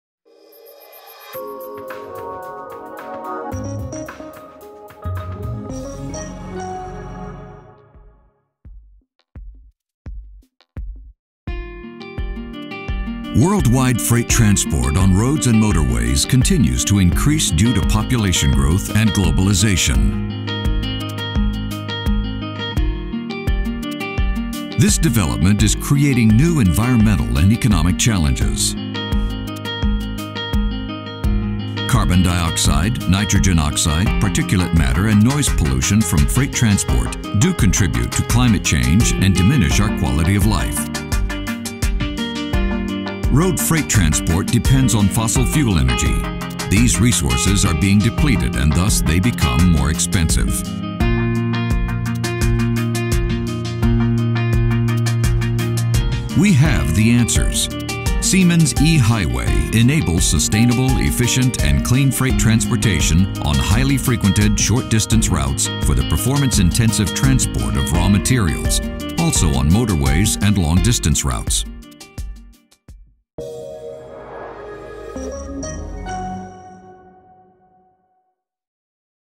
Siemens eHighway - Short Animated Video Intro : Electric Highway
Short animated video intro to present eHighway, the electric highway by Siemens.